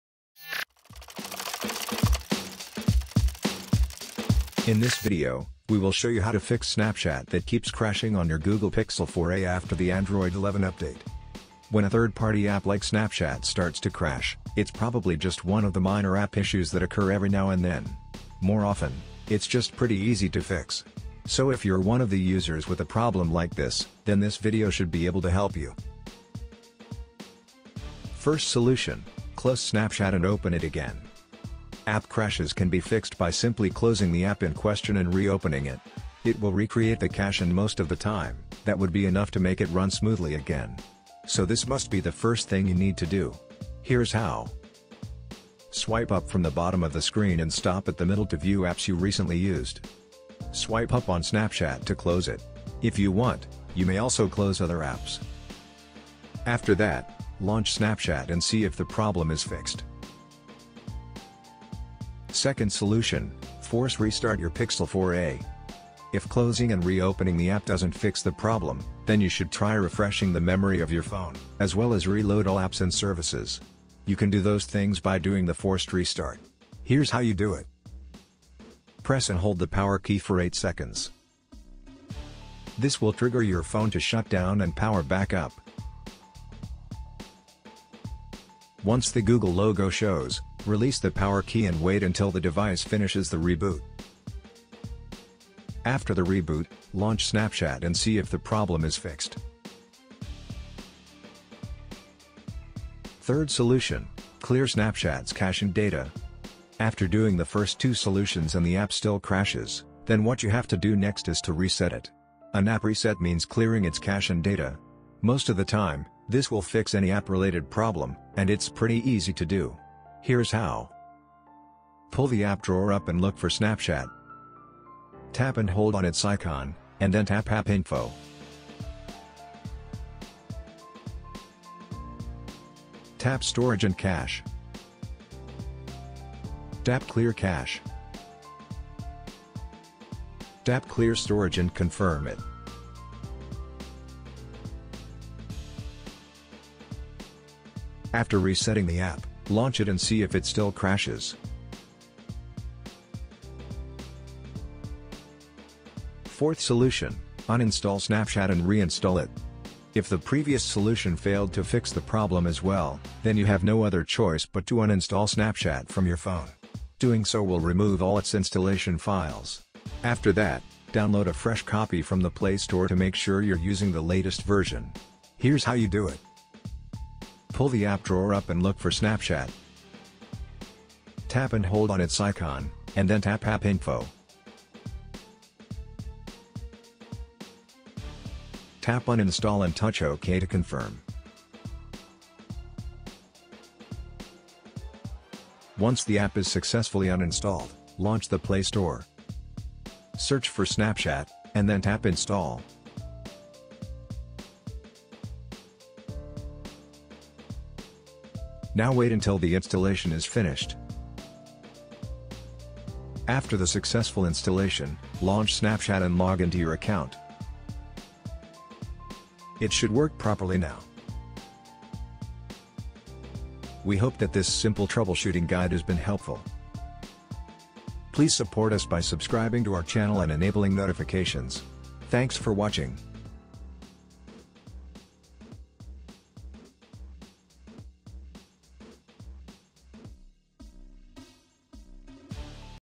How To Fix Snapchat That Keeps Crashing on Google Pixel 4a After Android 11
In this troubleshooting guide, we will show you what to do if Snapchat crashes on Pixel 4a after the Android 11 update. Issues like this are often minor and you may be able to get rid of it by doing a couple of procedures. Read on to learn more how to fix it.

When a third-party app like Snapchat starts to crash, it’s probably just one of the minor app issues that occur every now and then. More often, it’s just pretty easy to fix. So if you’re one of the users with a problem like this, then this video should be able to help you.